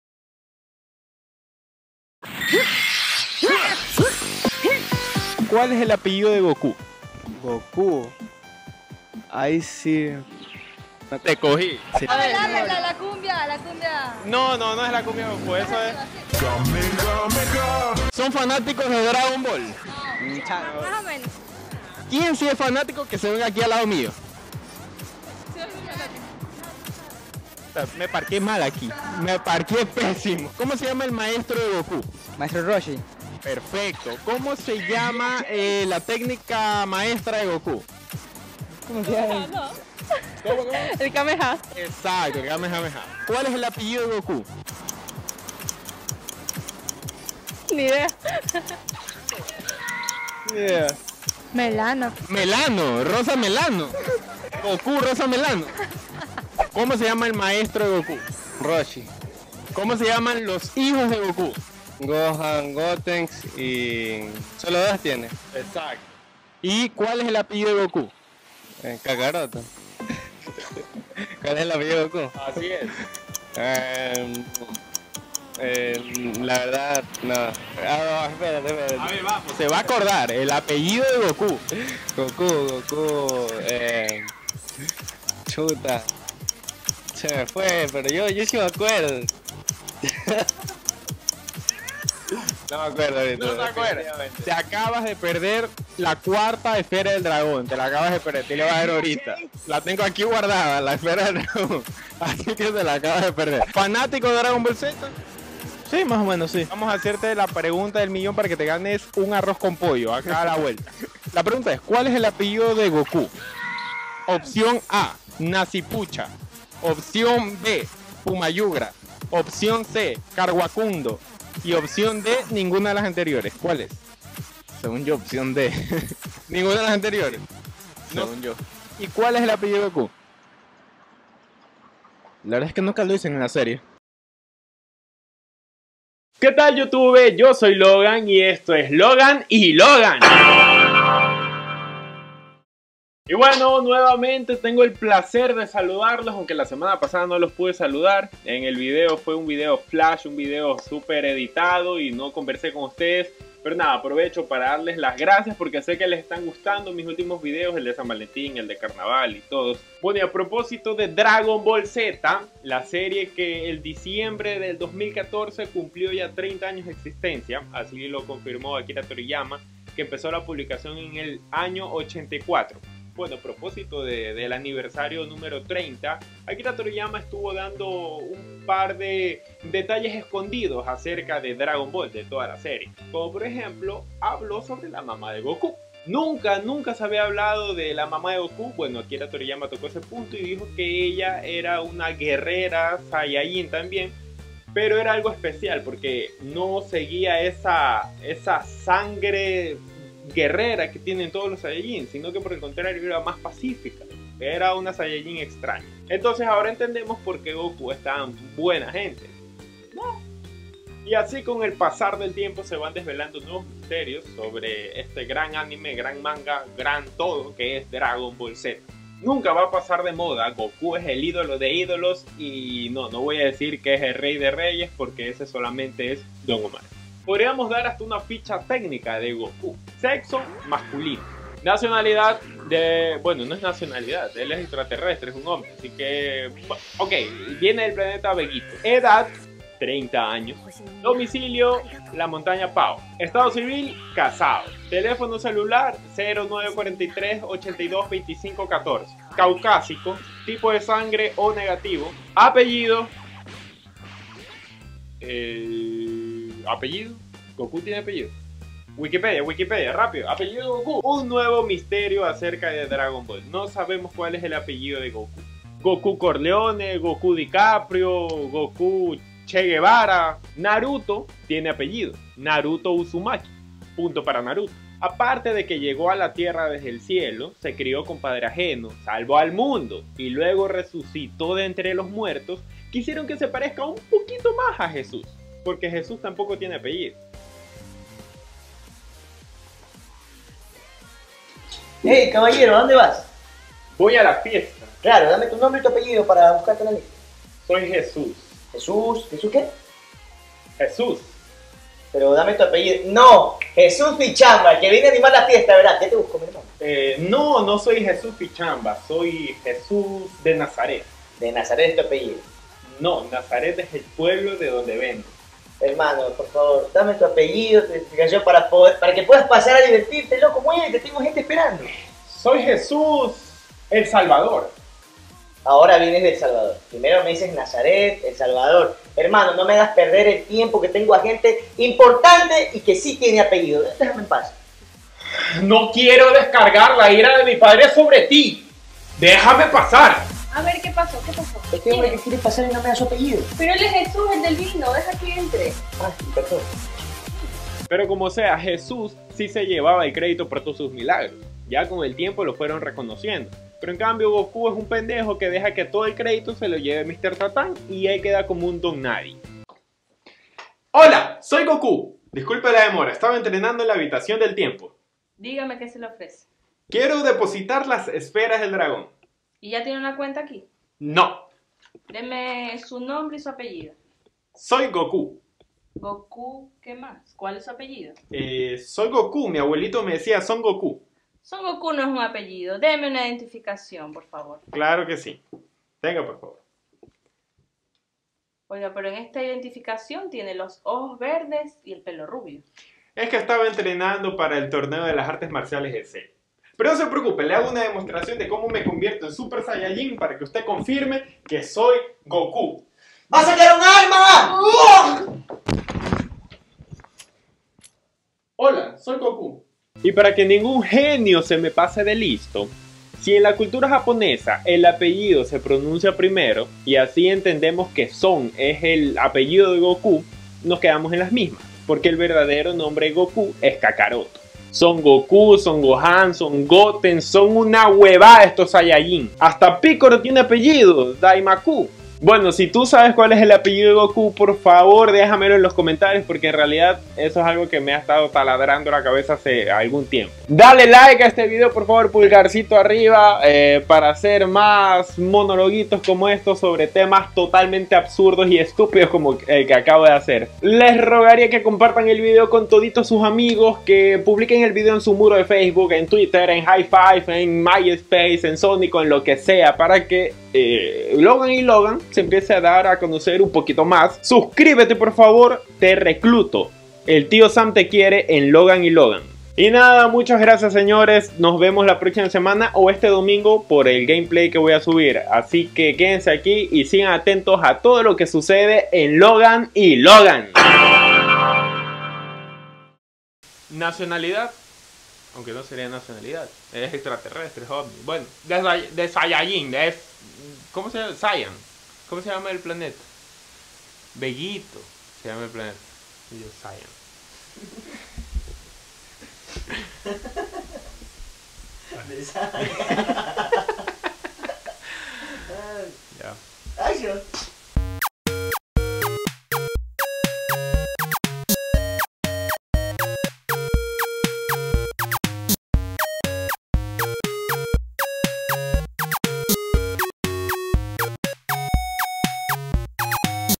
Dragon Ball Z: El apellido de Goku
Un misterio al que nunca nadie le paró bola. ¿Cual es el apellido de Goku? Quisiera reunir las esferas del dragón para preguntarle esto a Shenlon.
Like por mi peinado de Goku!!
Fav, comenta y comparte :3